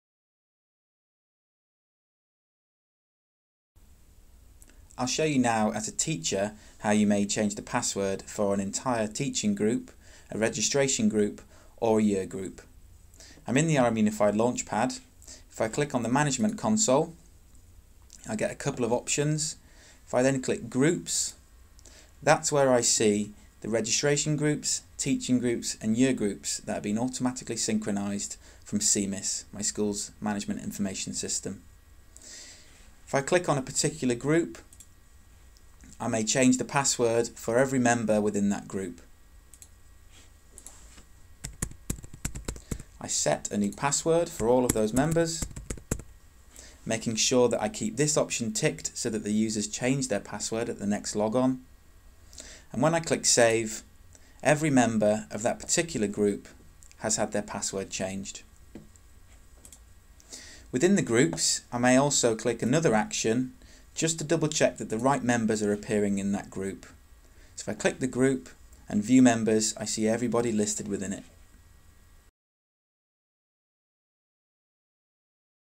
How to change passwords for entire groups in RM Unify - Tutorial
This video explains How to change passwords for entire groups in RM Unify.